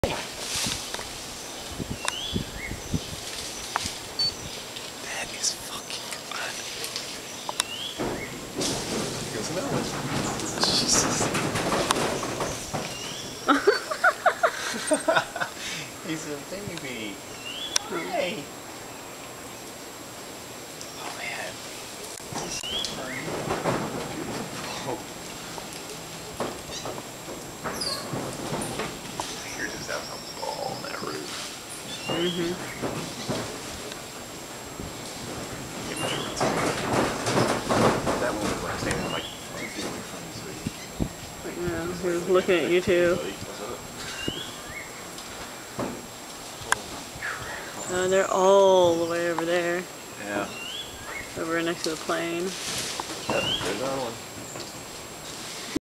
Manuel Antonio May 2009, Mono-Titi monkeys
Playful monkey's in the tree